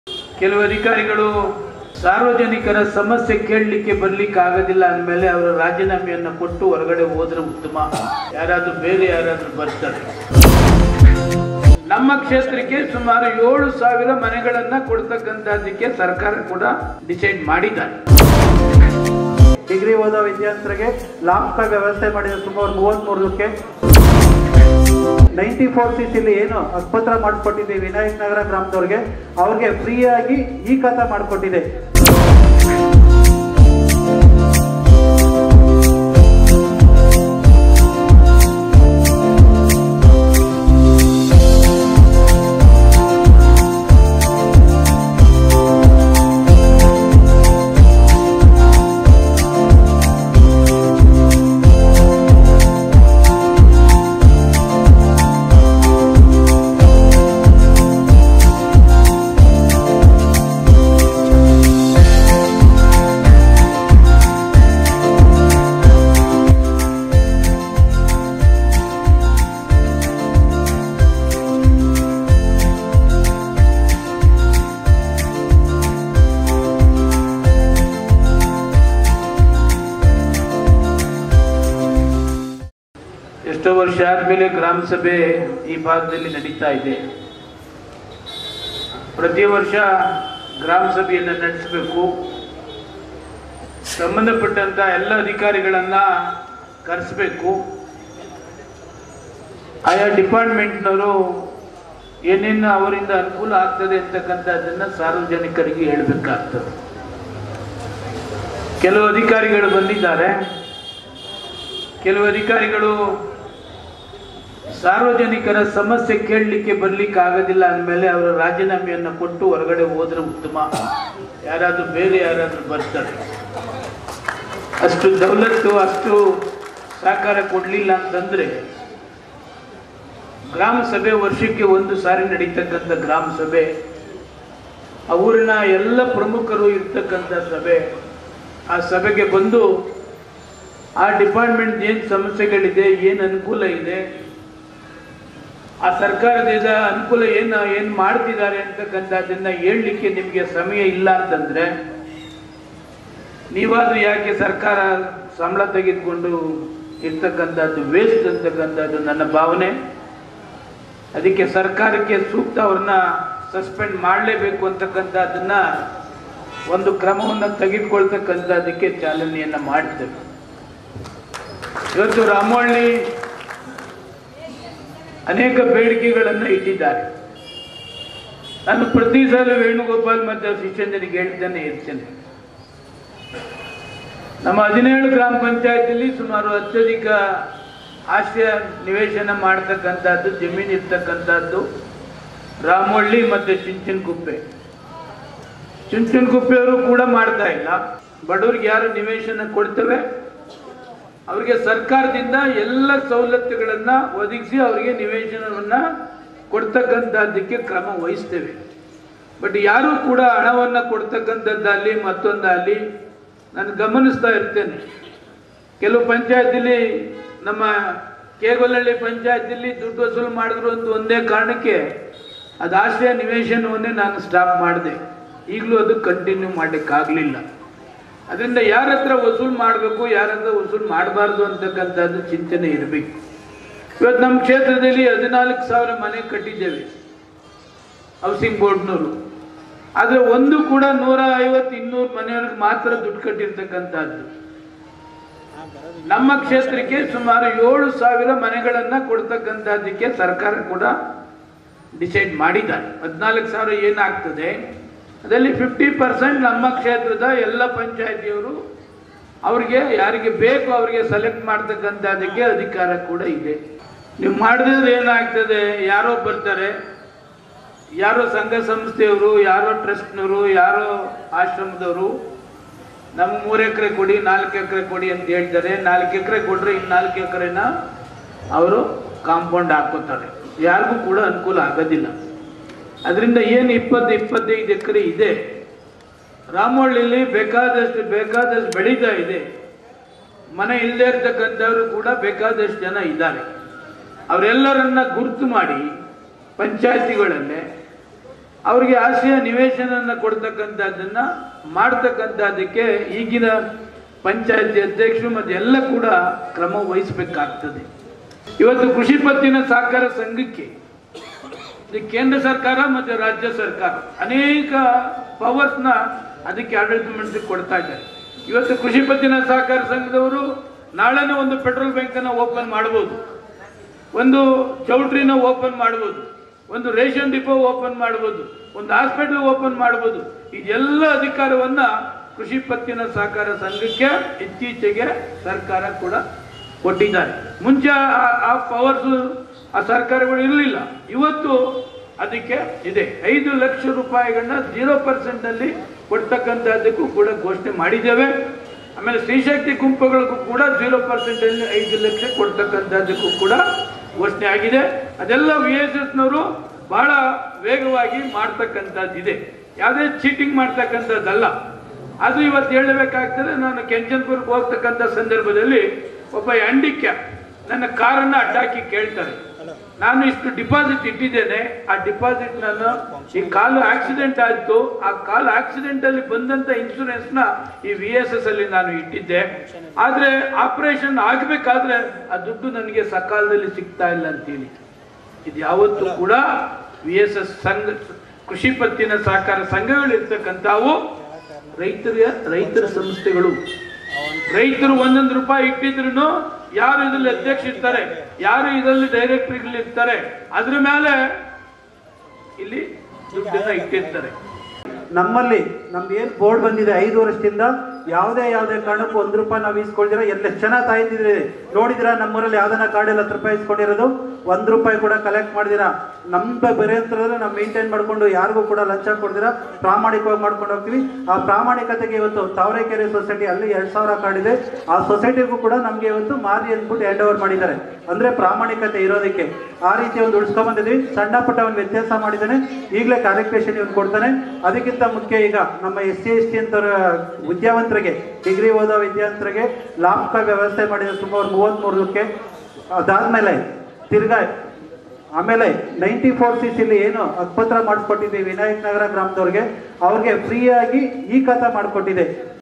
ST Somashekar : ಅಧಿಕಾರಿಗಳು ಸಾರ್ವಜನಿಕರ ಸಮಸ್ಯೆಗಳನ್ನ ಆಲಿಸದಿದ್ದರೆ ಅವರು ರಾಜಿನಾಮೆ ನೀಡಿ ಹೊರಡಬಹುದು.
ಯಶವಂತಪುರ ಕ್ಷೇತ್ರದ ರಾಮೋಹಳ್ಳಿ ಗ್ರಾಮ ಪಂಚಾಯತಿಯಲ್ಲಿ ವಾರ್ಷಿಕ ಸಭೆ ನಡೆಸಲಾಯಿತು. ಈ ಸಂದರ್ಭದಲ್ಲಿ ಶಾಸಕ ಎಸ್. ಟಿ. ಸೋಮಶೇಖರ್ ಕಾರ್ಯಕ್ರಮ ಉದ್ಘಾಟಿಸಿದರು. ಅಧ್ಯಕ್ಸತೆಯನ್ನು ವೇಣುಗೋಪಾಲ್ ವಹಿಸಿದ್ದರು. ಇದೆ ಸಂದರ್ಭದಲ್ಲಿ ವಿವಿಧ ಸರ್ಕಾರೀ ಸವಲತ್ತುಗಳನ್ನು ಸಾರ್ವಜನಿಕರಿಗೆ ವಿತರಣೆ ಮಾಡಿದರು. ಉಚಿತ ಲ್ಯಾಪ್ಟಾಪ್, ಈ ಸ್ವತ್ತು ಪತ್ರಗಳನ್ನು ವಿತರಿಸಿದರು.